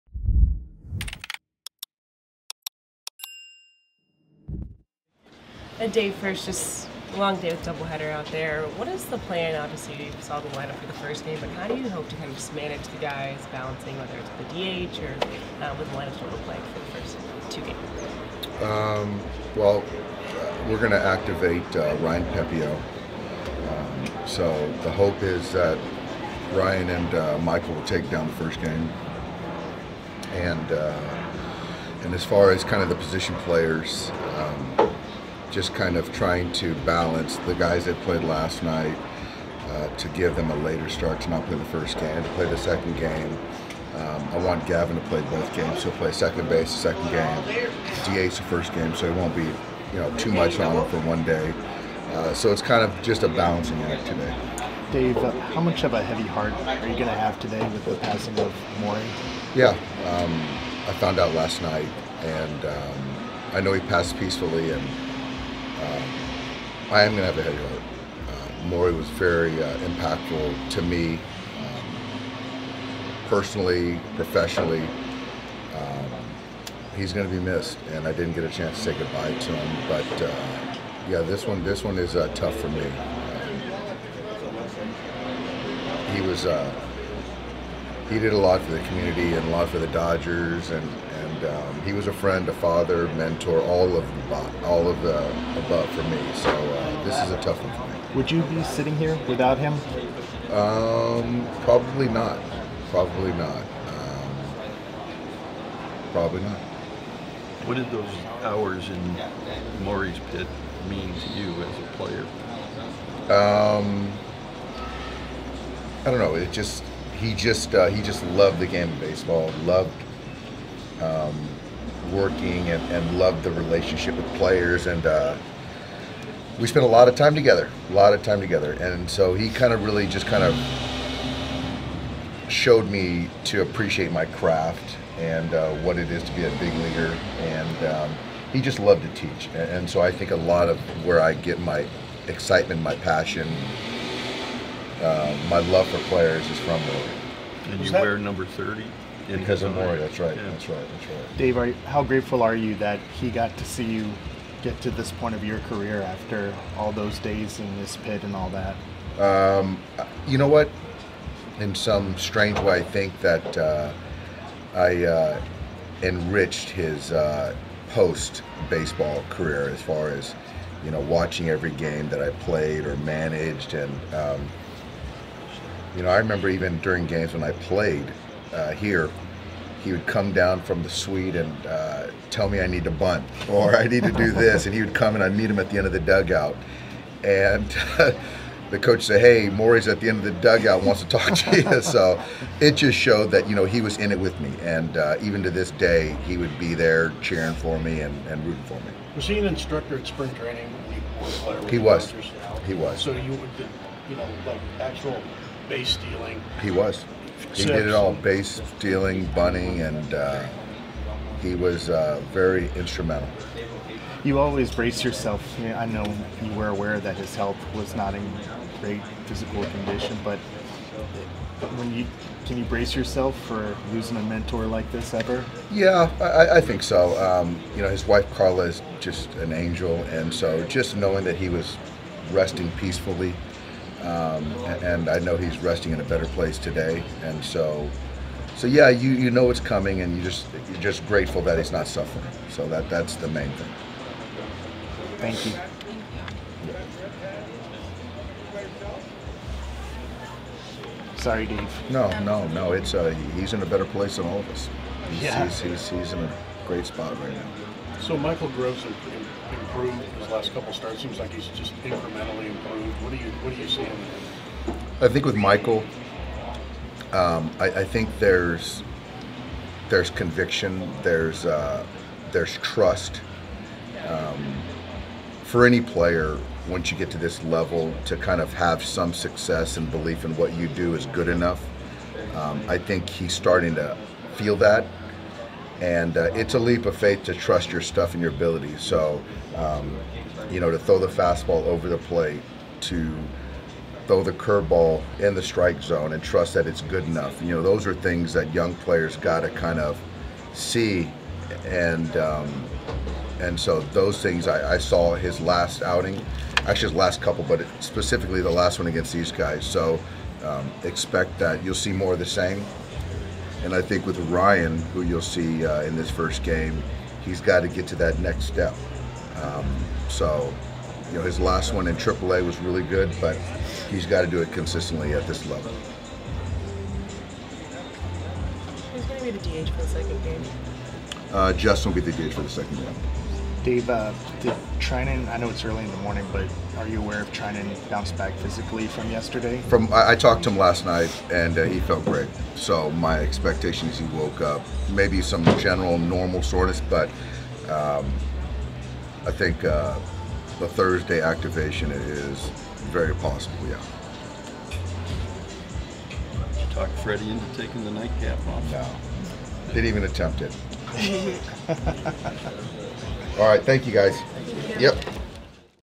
Dodgers pregame: Dave Roberts mourns passing of Maury Wills
Los Angeles Dodgers manager Dave Roberts reflected on his relationship with Maury Wills, who passed away at 89 years old.